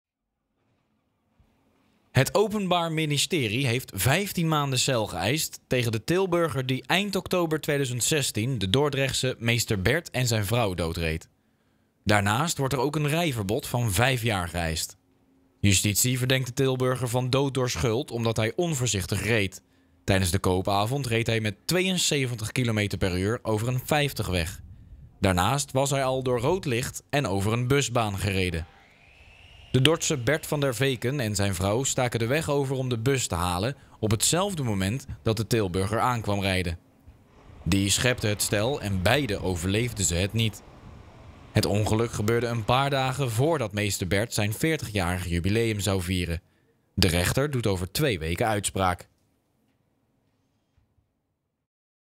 15 maanden cel geëist voor doodrijder meester Bert
NI20180615_MeesterBert